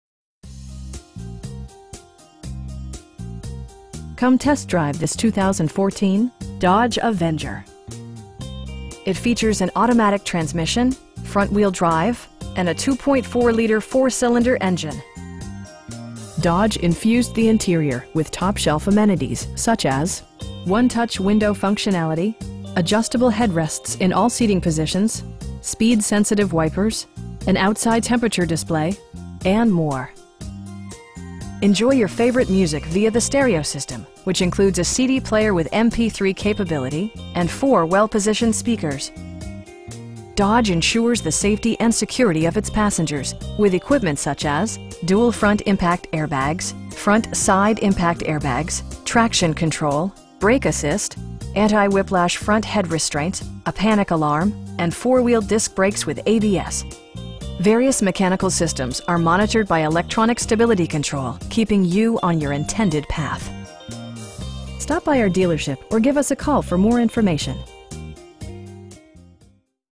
2014 Dodge Avenger SE in Duluth, GA 30096
Come test drive this 2014 Dodge Avenger. It features an automatic transmission, front-wheel drive, and a 2.4 liter 4 cylinder engine. Dodge infused the interior with top shelf amenities, such as: 1-touch window functionality, adjustable headrests in all seating positions, speed sensitive wipers, an outside temperature display, tilt and telescoping steering wheel, cruise control, and more. Dodge ensures the safety and security of its passengers with equipment such as: dual front impact airbags, front SIDE impact airbags, traction control, brake assist, anti-whiplash front head restraints, a panic alarm, and 4 wheel disc brakes with ABS. Various mechanical systems are monitored by electronic stability control, keeping you on your intended path. We pride ourselves in consistently exceeding our customer's expectations. Stop by our dealership or give us a call for more information.